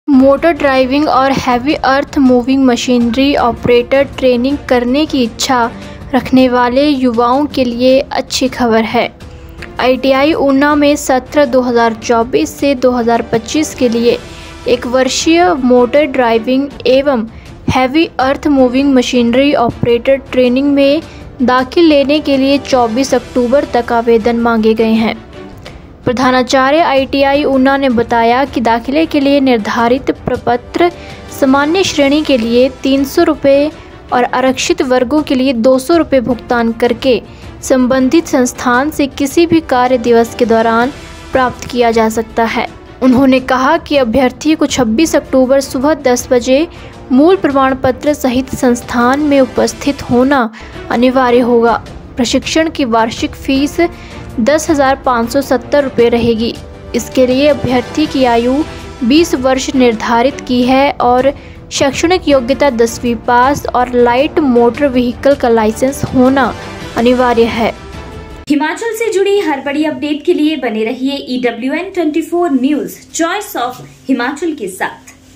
ऊना : हेवी अर्थ मूविंग मशीनरी ऑपरेटर ट्रेनिंग, 24 अक्टूबर तक करें आवेदन
There is good news for the youth who wish to do motor driving and heavy earth moving machinery operator training. Applications have been invited till October 24 for admission in one-year motor driving and heavy earth moving machinery operator training for the session 2024-25 at ITI Una.